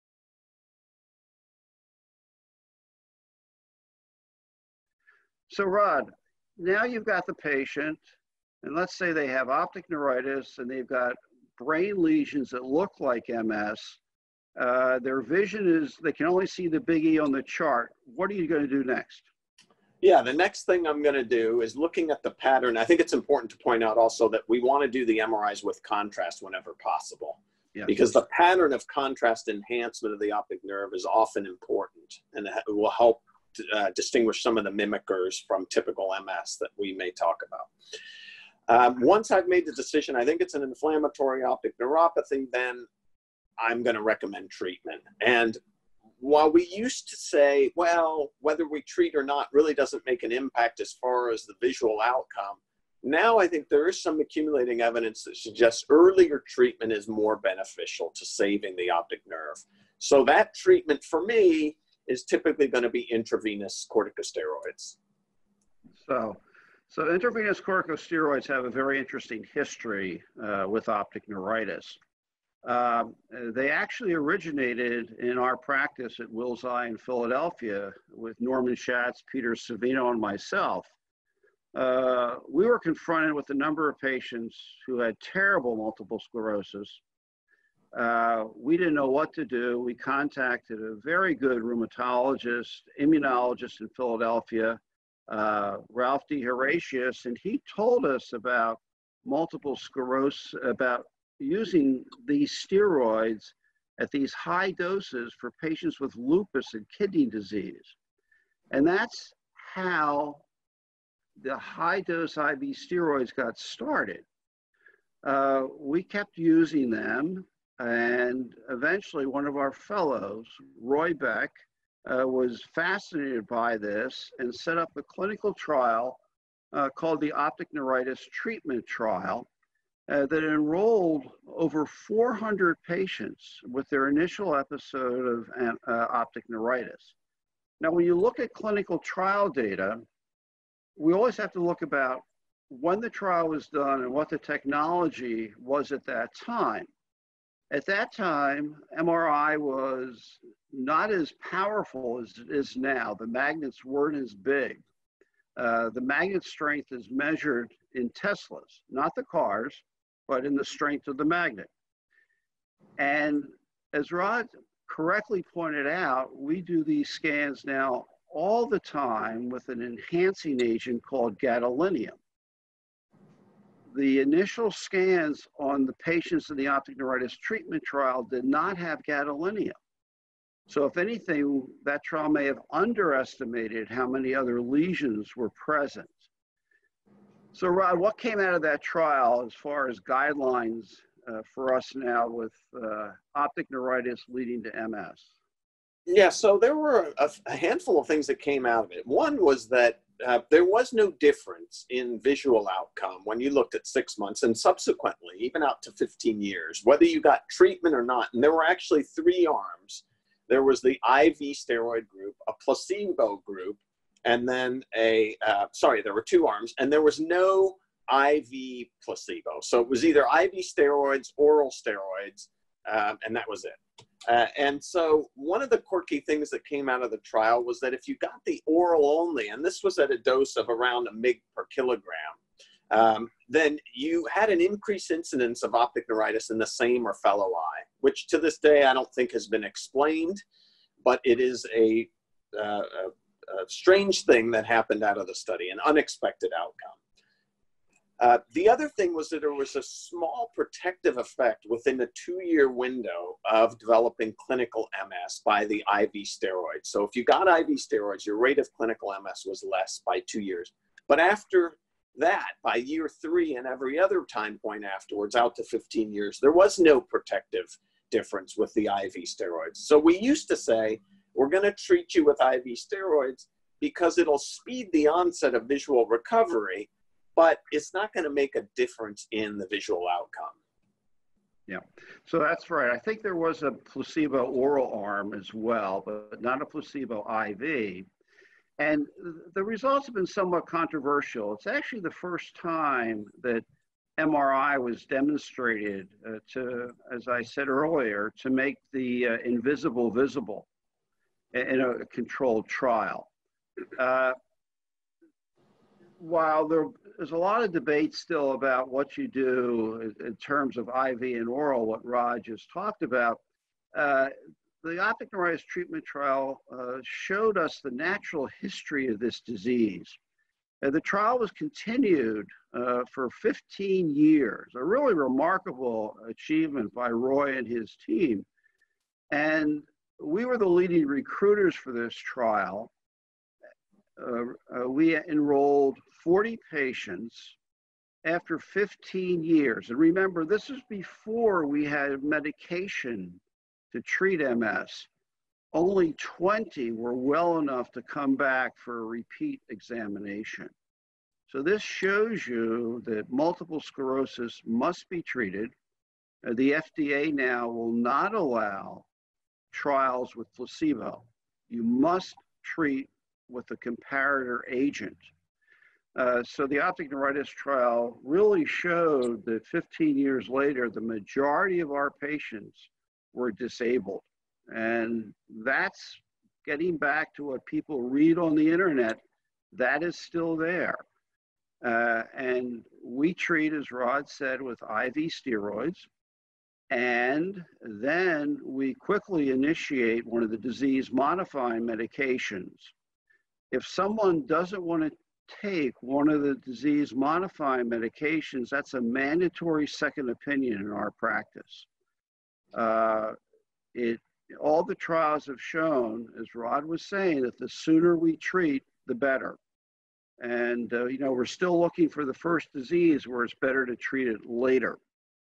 Optic Neuritis and Brain Lesions
A discussion on best practices of patients with optic neuritis who present with brain lesions and a pivotal MRI trial and its impact on the treatment paradigm.